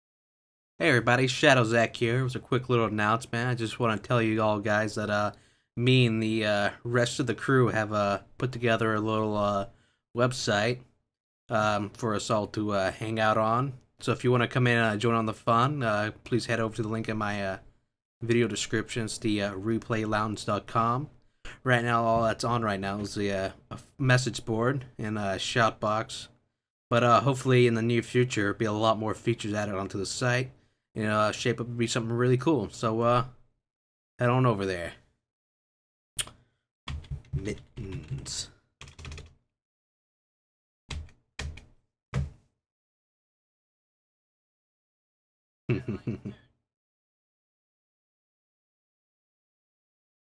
"The Replay Lounge" New site launched with "The Crew"
Me and The rest of the crew have been hard at work over the past few weeks to create and launch a new site where we can all hang out at. Register and Join the fun!! .